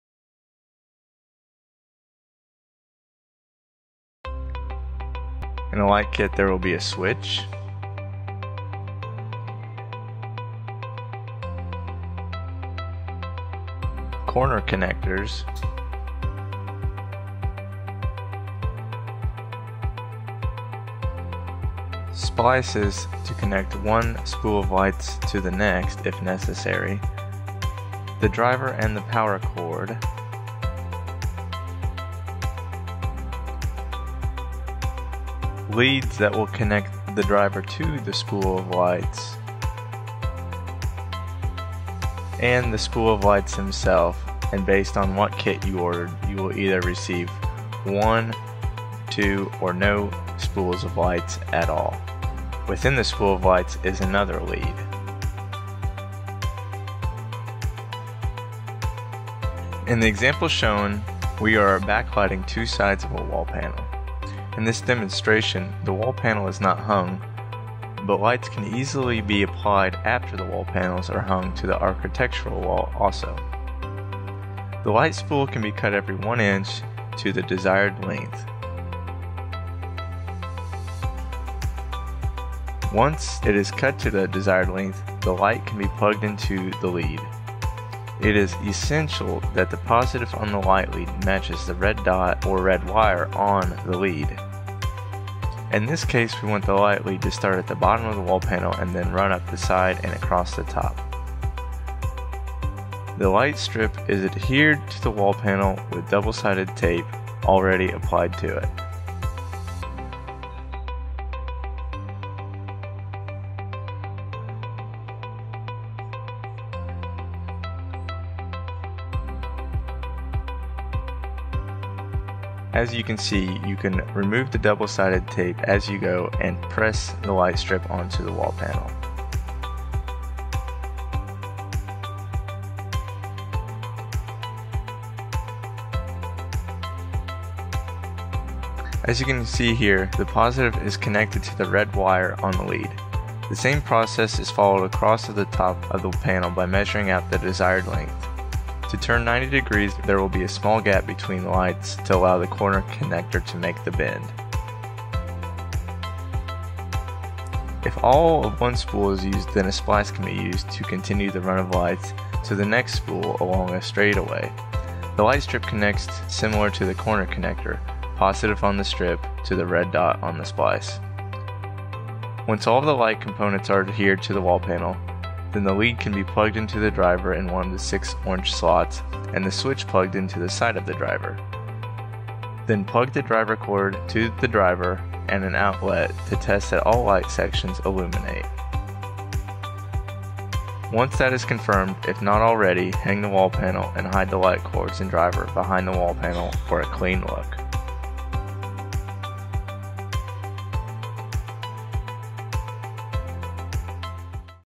Tessera Installation - Wall Panel Accent Lighting
Installation instructions for Tessera Wall Panel Accent Lighting by National.

0:05 - Verify Accent Lighting Parts
1:00 - Cut Light Strip to Desired Length
1:26 - Attach Light Strip to Power Lead
1:44 - Adhere Light Strip to Wall Panel
2:19 - Verify Light Strip is Connected to Proper Power Terminal
2:38 - Splices can be Used to Connect More Light Strips
2:55 - Verify Light Strip Works before Installing